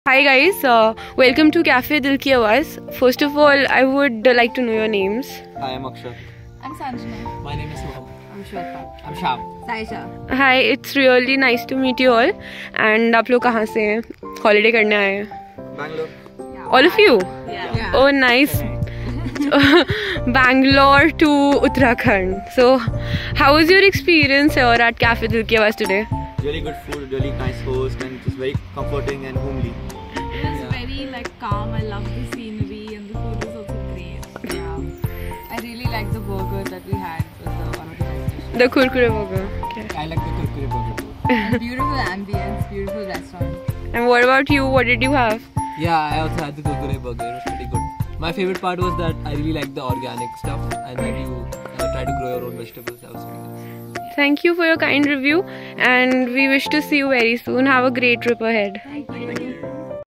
Experience your Home Kitchen over a Holiday @ Cafe Dil Kii Awaaz
Experience your Home Kitchen even over a Holiday @ Cafe Dil Kii Awaaz, Signature Dishes by Shreeya Katyal at Maldevta Sahastradhara Bypass Road, Pustadi Gaon, Dehradun.